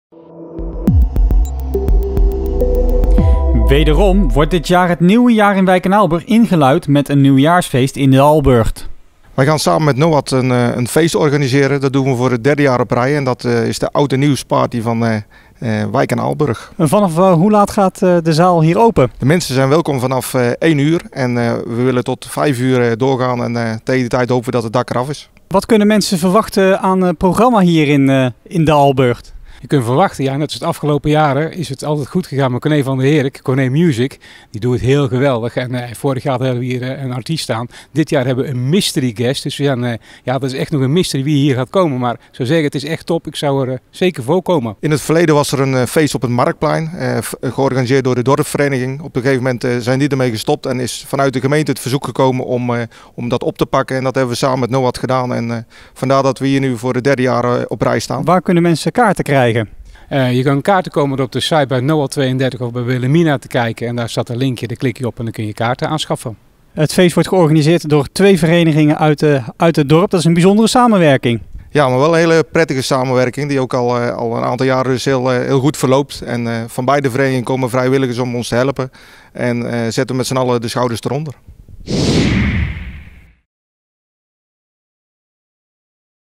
Wederom oud-en-nieuwfeest in Wijk en Aalburg - Altena TV
WIJK EN AALBURG - In Wijk en Aalburg wordt het nieuwe jaar ingeluid met een nieuwjaarsfeest in d’Alburcht. De deuren gaan in de nieuwjaarsnacht om 01:00 uur open. Iedereen kan elkaar in d’Alburcht ontmoeten om elkaar alle goeds te wensen en te proosten op een nieuw jaar.

Het feest wordt georganiseerd door de twee voetbalverenigingen van Wijk en Aalburg. Namelijk NOAD ’32 en Wilhelmina ’26. Deze avonden zijn er gezellige dj’s en een mystery guest.

Dit feest is voor alle leeftijden, dus iedereen is welkom. Maar vergeet niet je legitimatie mee te nemen. Dit jaar zullen er geen bussen rondrijden om feestvierders op te halen.
Kaarten zijn in de voorverkoop verkrijgbaar op de websites van Wilhelmina ’26 en NOAD ’32.